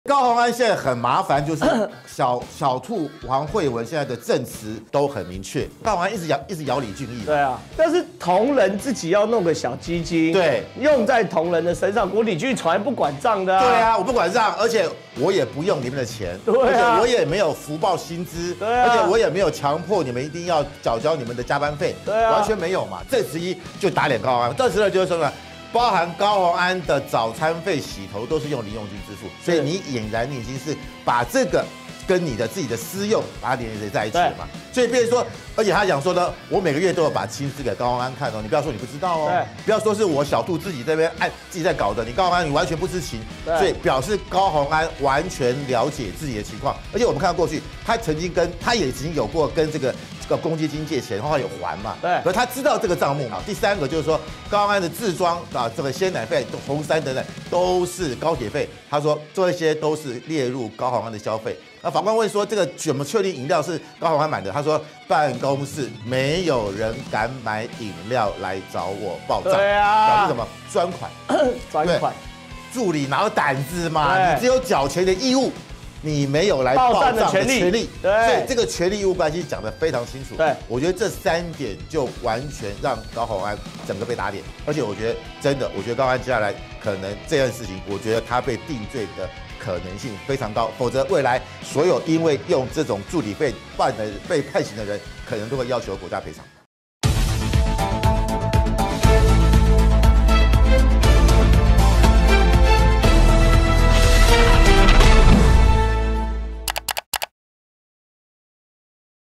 證人小兔提三關鍵證詞打臉 范世平：高虹安被定罪機率高！
主持人：李正皓
來賓：民進黨政策會執行長　王義川｜政治學者　范世平｜時事評論員　溫朗東

鋒燦傳媒FTNN新聞網．傳遞真相的聲音
　粉專  / newsftnn